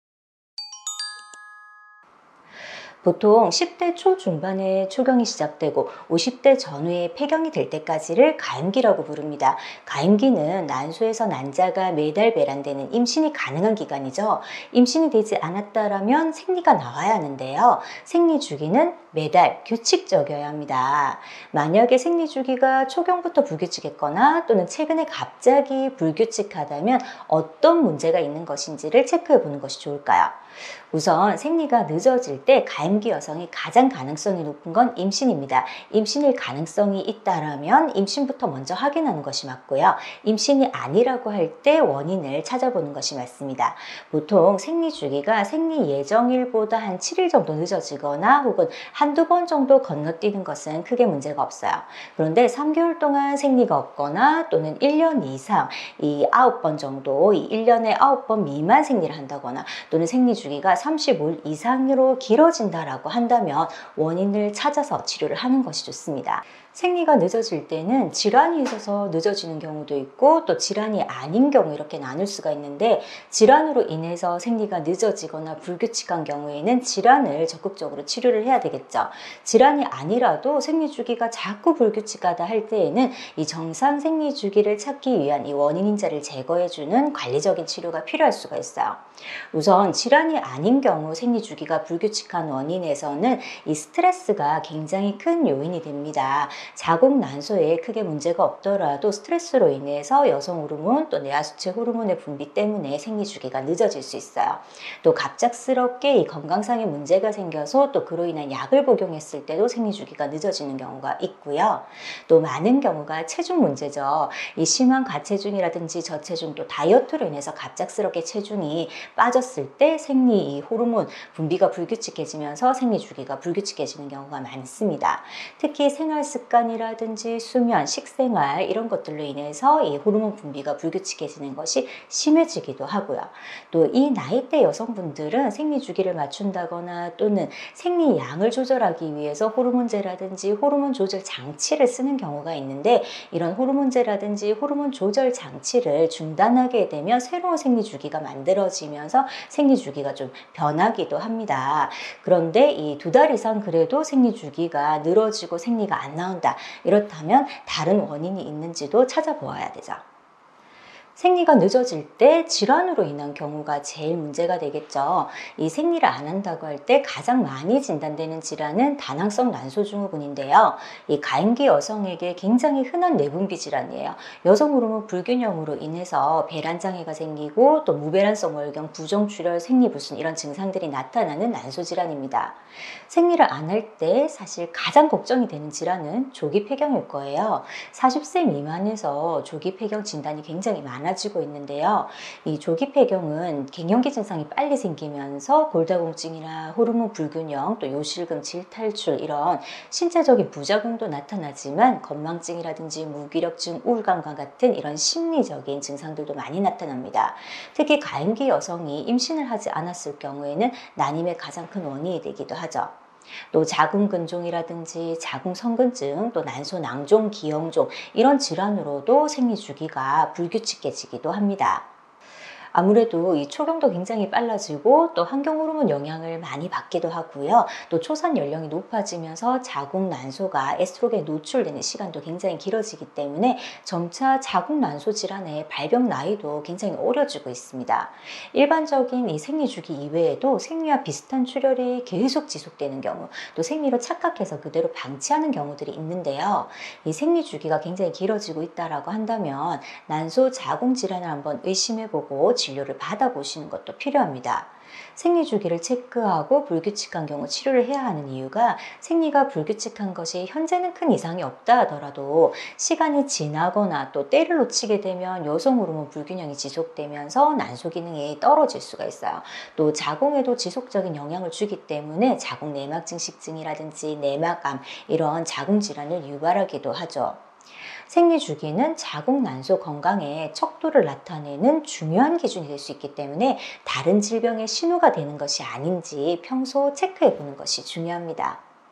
생리가 안나온다면? 생리가 안나올 때 해당되는 모든 경우의 수를 확인해보자
가임기 여성의 경우 매달 생리를 해야 하는데요.
생기가 가끔 건너뛰는 것은 큰 문제는 없지만
자주 주기가 맞지 않거나
무월경이 지속되거나
특발성으로 생리주기가 길어질 때

무엇이 원인인지 또 어떤 질환들을 주의해야 하는지 알아봅시다.